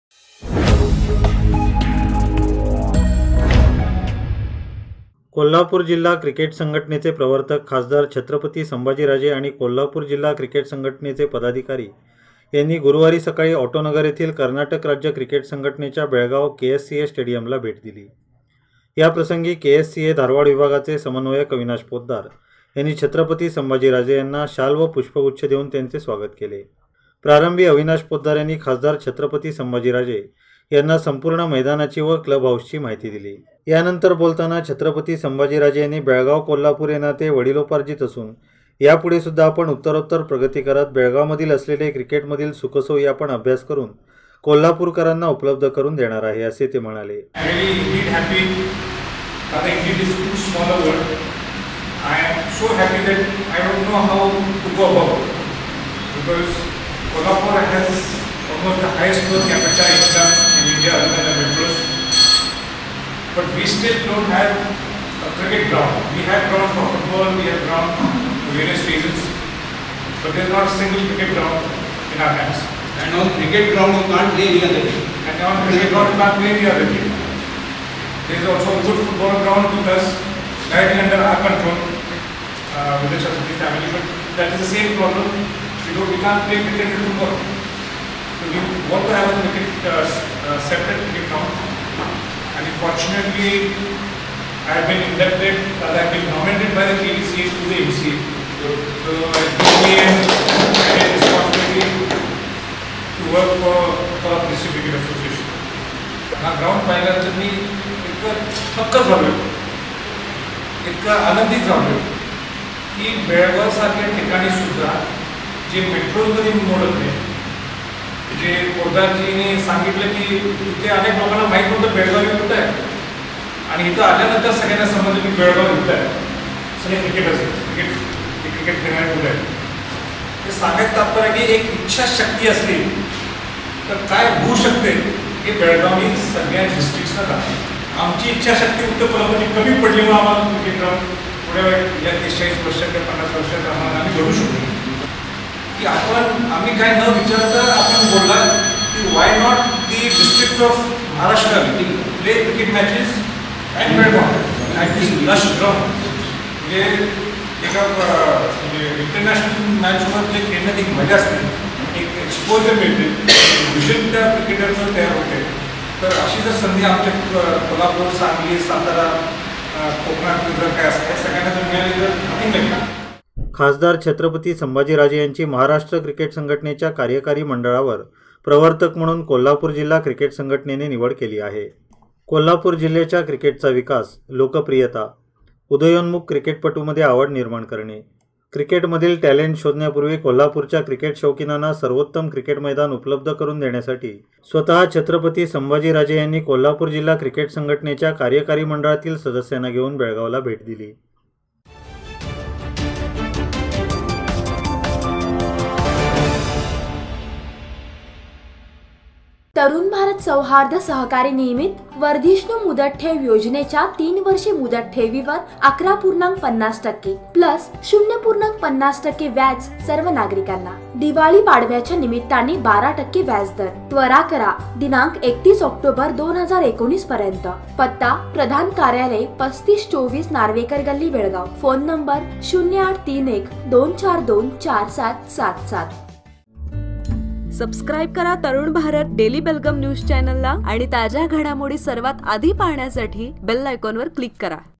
कोल्हापुरात संघटनेचे स्वत:च्या मालकीचे मैदान बनविण्याचे पहिले टार्गेट | Belgaum News | 31-10-2019
कोल्हापुर जिल्हा क्रिकेट संघटनेचे प्रवर्तक खासदार छत्रपती संभाजी राजे आणि कोल्हापूर जिल्हा क्रिकेट संघटनेचे पदाधिकारी यांनी गुरूवारी सकाळी ऑटोनगर येथील कर्नाटक राज्य क्रिकेट संघटनेच्या बेळगाव केएससीए स्टेडियमला भेट दिली.

Ads :